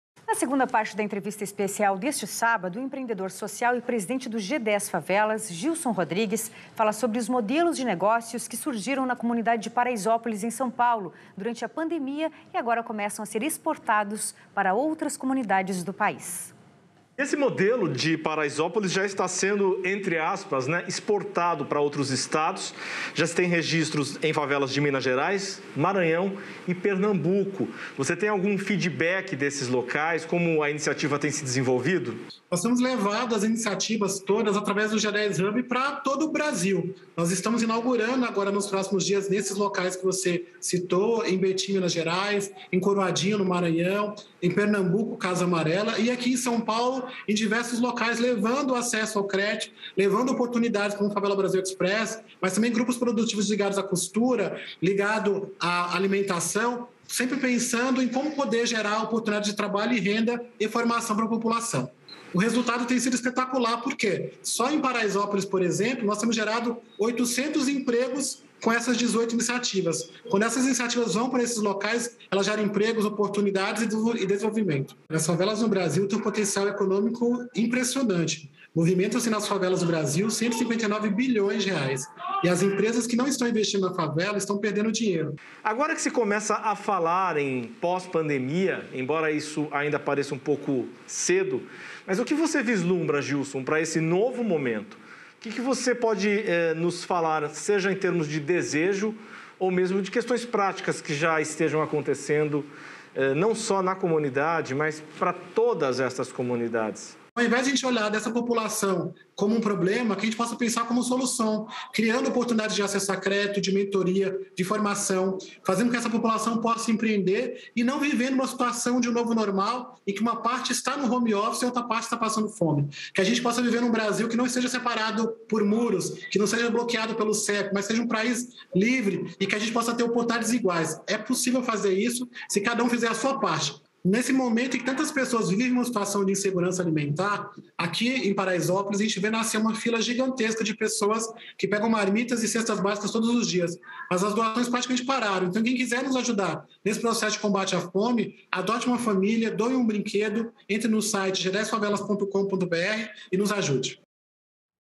“Empresas que não investem na favela estão perdendo dinheiro”, diz presidente do G10 Favelas
Em entrevista exclusiva ao JornaldaCultura, Gilson Rodrigues, presidente do G10 Favelas, contou como foi o comportamento dos moradores das comunidades de todo o país para enfrentar a pandemia da Covid-19. As favelas foram fortemente afetadas pelo coronavírus e foi necessário criar projetos para socorrer os moradores. Veja como essas propostas foram desenvolvidas e como elas ajudar as pessoas com menos renda.